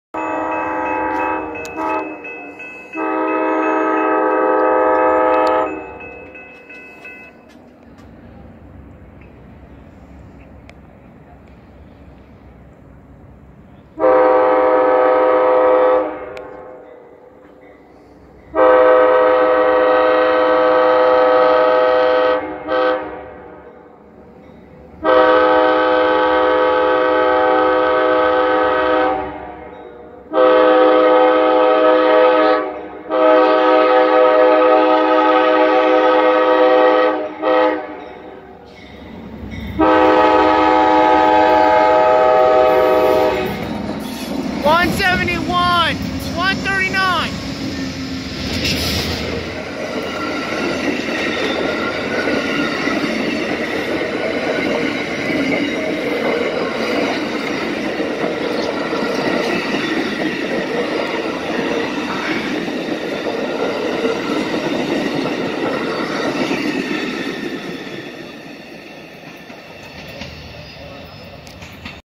Amtrak 20 "The Crescent" in Norcross, GA - 2/13/19
AMTK 171 leads February 13th's Amtrak 20 "The Crescent".